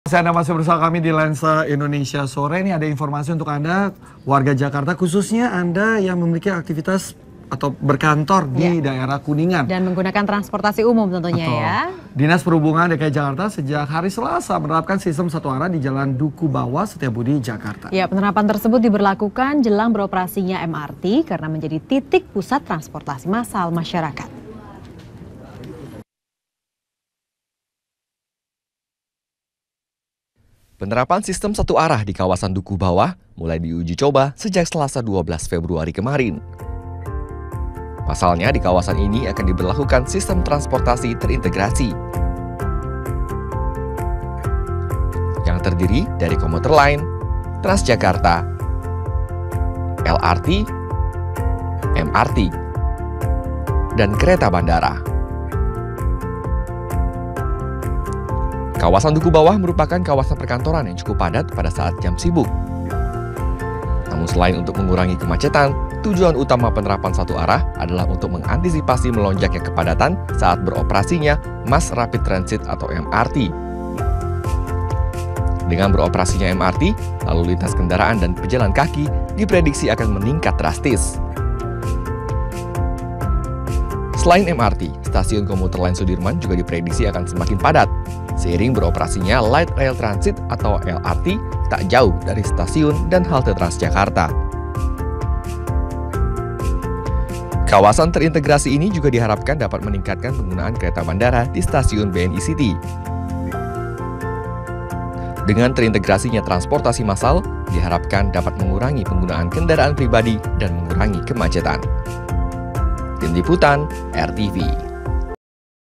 Integrasi Transportasi Massal di Kawasan Jalan Sudirman, Jakarta
Integrasi transportasi massal di kawasan Jalan Sudirman, Jakarta akan segera diterapkan yang terdiri dari Commuter line, Transjakarta, LRT , MRT, dan kereta bandara.